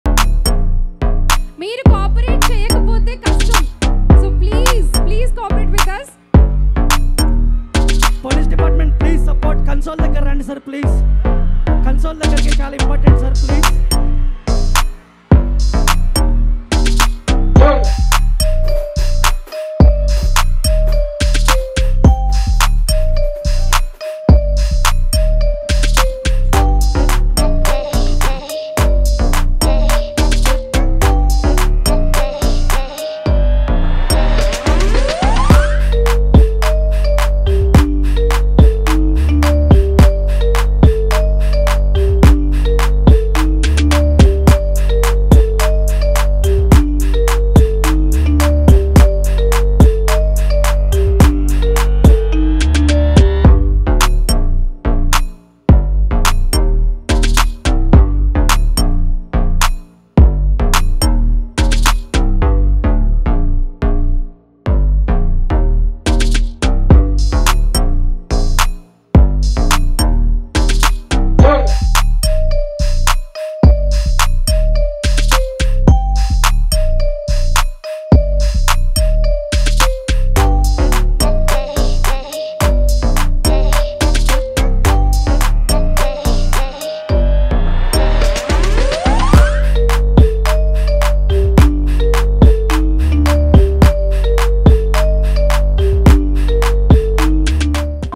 Hero Ram Pothineni took care of His Heroine Krithi Shetty at The Warriorr Theatrical Trailer Launch
tfpc tollywoodnews entertainment Hero Ram Pothineni took care of His Heroine Krithi Shetty at The Warriorr Theatrical Trailer Launch